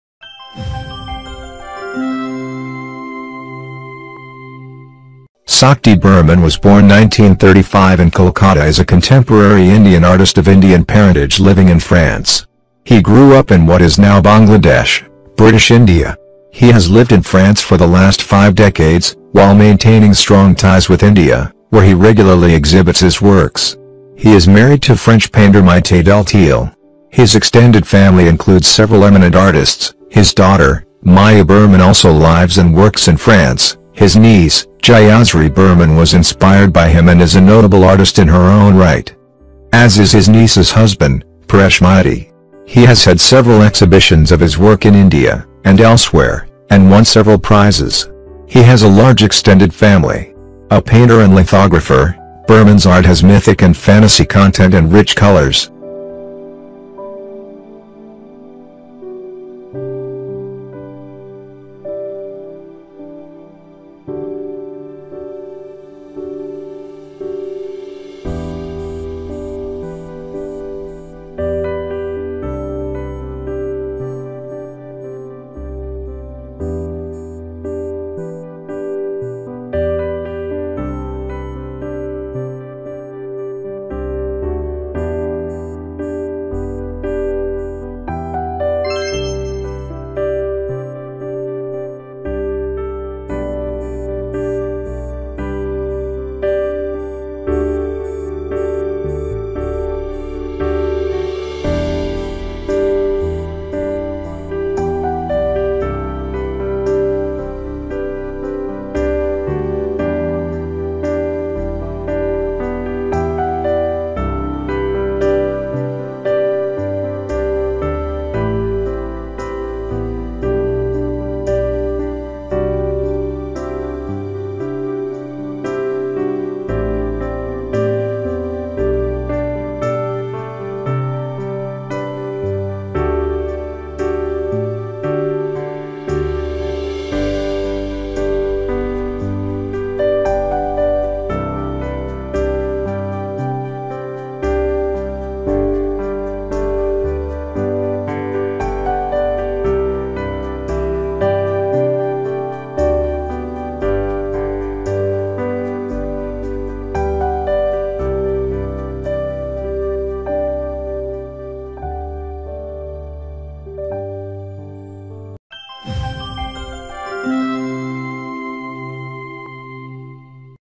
Artist Sakti Burman | Painting | WAA
Sakti Burman was born 1935 in Kolkata is a contemporary Indian artist of Indian parentage living in France. He grew up in what is now Bangladesh, British India. He has lived in France for the last five decades, while maintaining strong ties with India, where he regularly exhibits his works. He is married to French painter Maite Delteil. His extended family includes several eminent artists, his daughter, Maya Burman also lives and works in France, his niece, Jayasri Burman was inspired by him and is a notable artist in her own right, as is his niece's husband, Paresh Maity. He has had several exhibitions of his work in India, and elsewhere, and won several prizes. He has a large extended family. A painter and lithographer, Burman's art has mythic and fantasy content and rich colors.